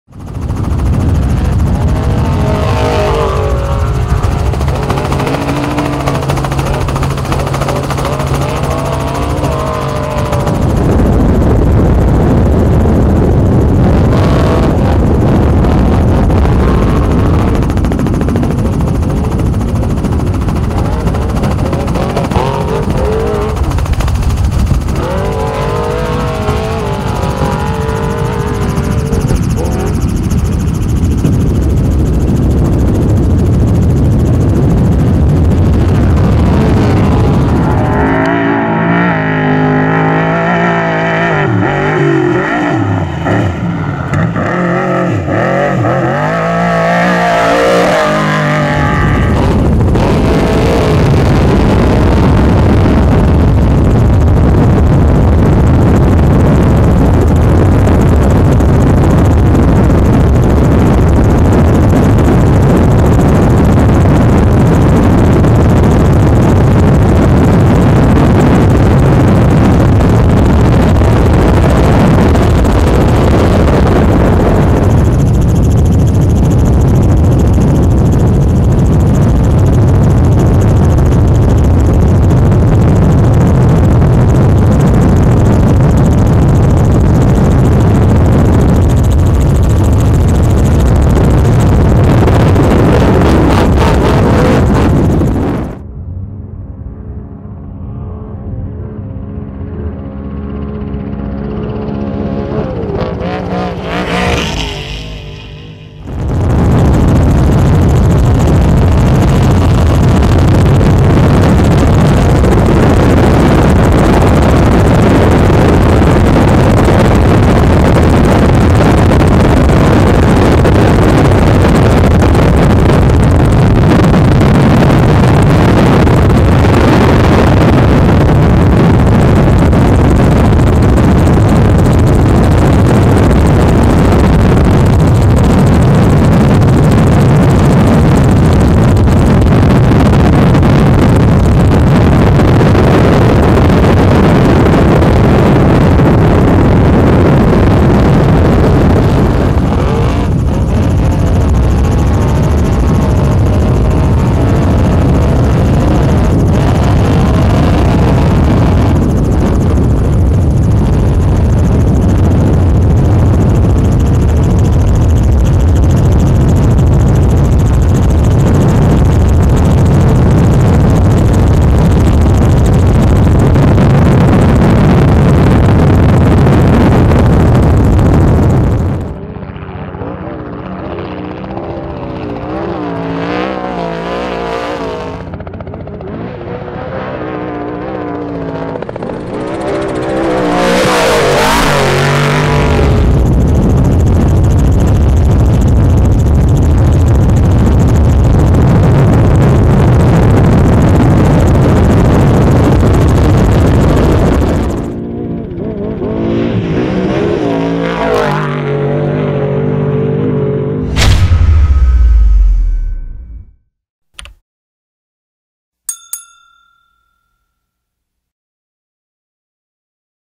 Chasing Rob MacCachren || Parker 425 || Raw Footage
Raw Footage of Off-Road Racing Legend Rob MacCachren during the 2018 Parker 425. Enjoy